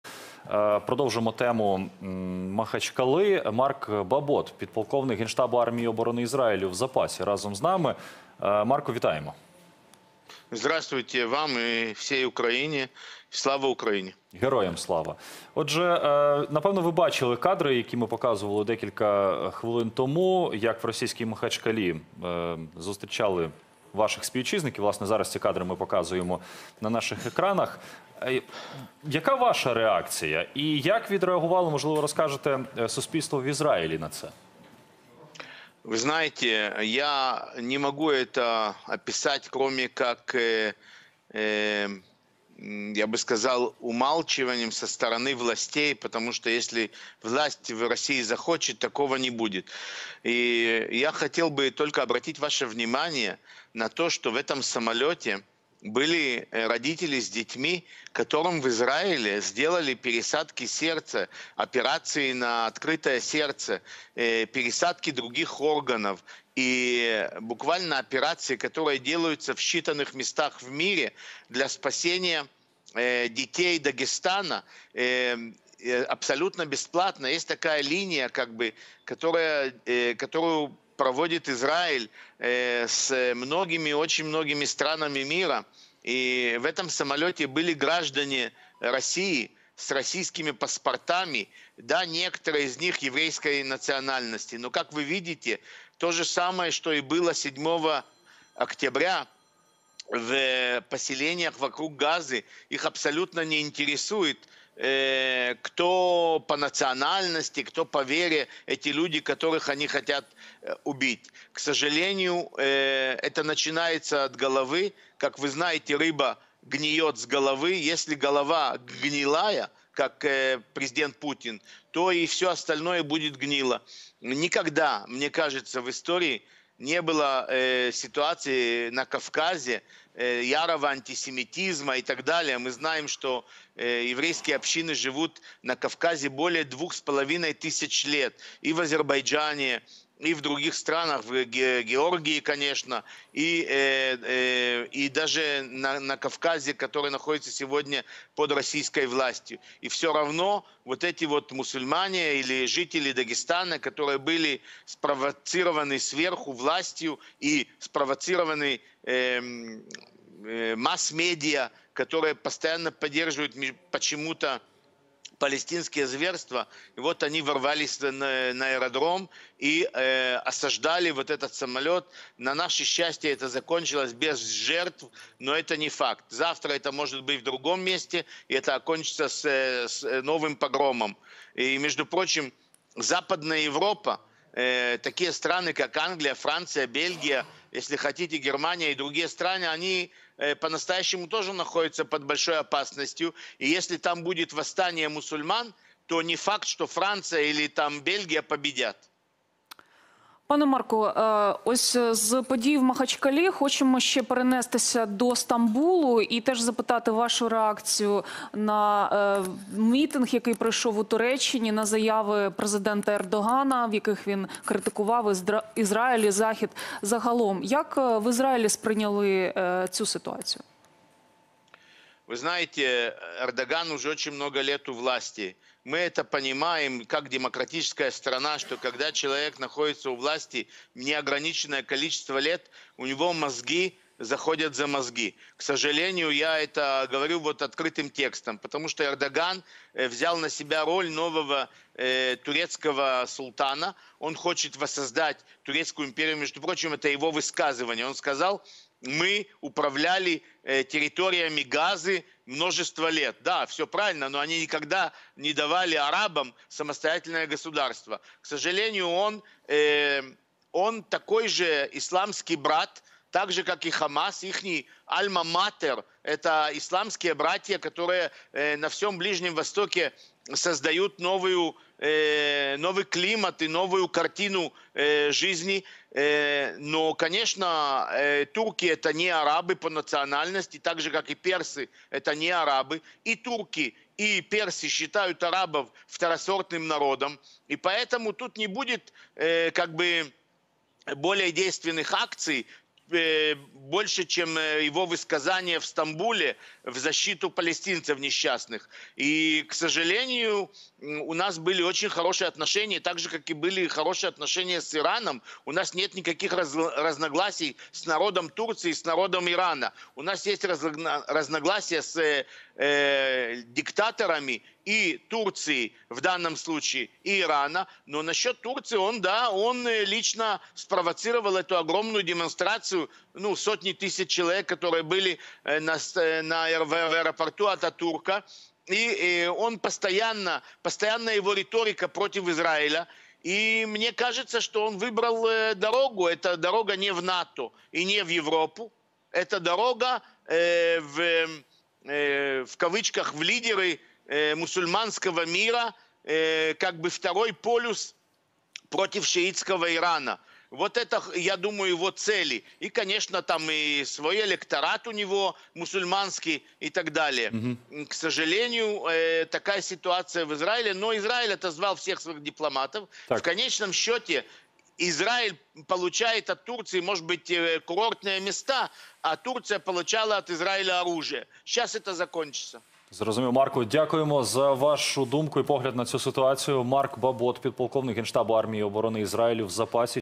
ВОНИ ПРОСТО ХОЧУТЬ ВБИВАТИ! БОБОТ: влада РФ спровокувала народ МАХАЧКАЛИ на такі ПОГРОМИ
Марк Бабот, підполковник штабу армії Ізраїлю в запасі про підтримку Туреччини ХАМАСУ та заворушення в аеропорту Махачкали.